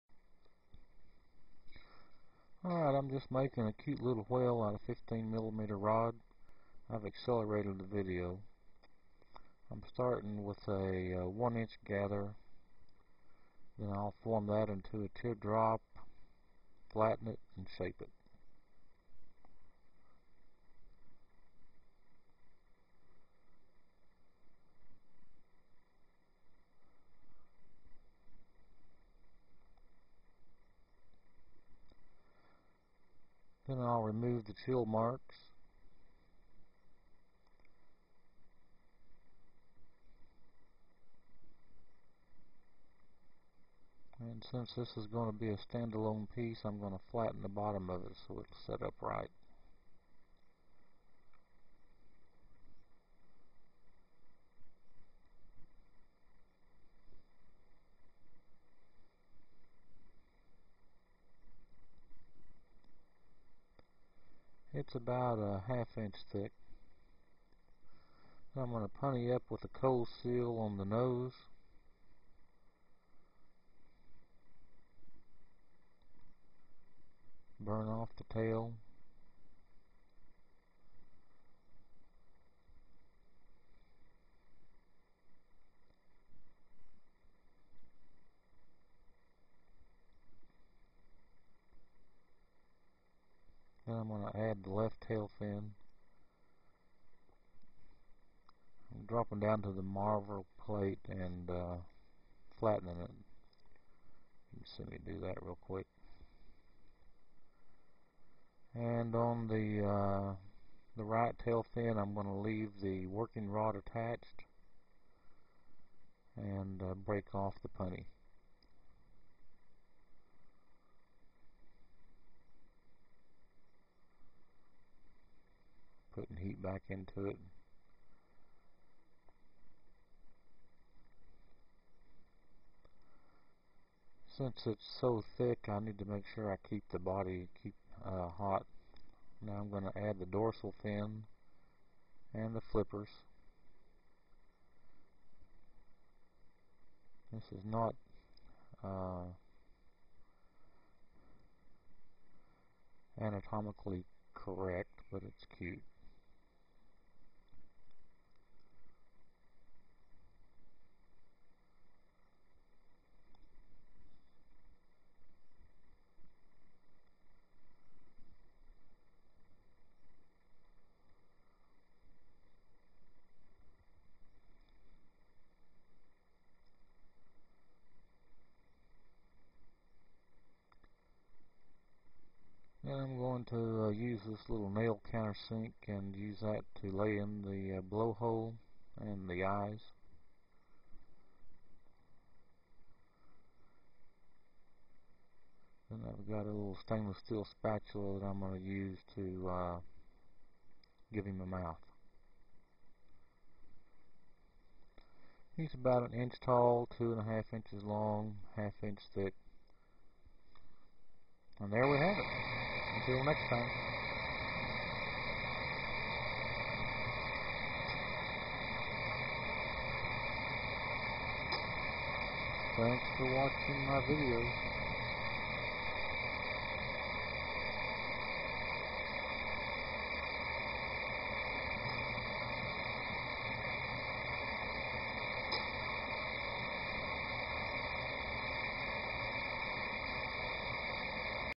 Whale
Cute little solid glass whale made from 15mm borosilicate.